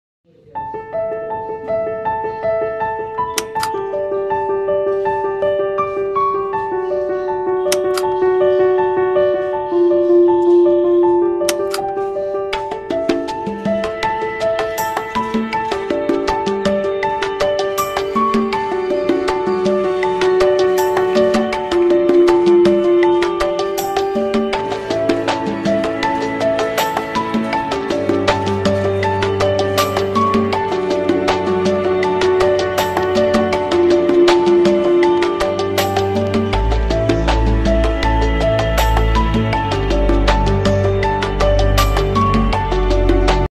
💯Volvo engine ka fain belt ka all pully# shorts mechanic pardeep
ye video me volvo truck dampar ka fan belt ka sara pully jo ki kitna hain ye aap es video me dekh sakte hain#shorts

mechanic pardeep#